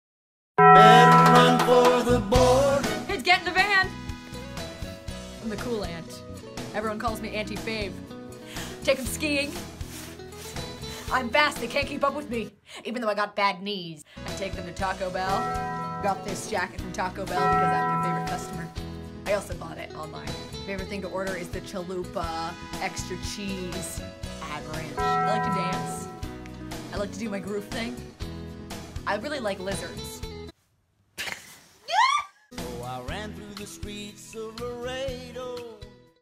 RAZOR BURN COMEDY: IMPROV Videos
Character Improv Sketch:
Meet Pam, the cool aunt ;) But first, Taco Bell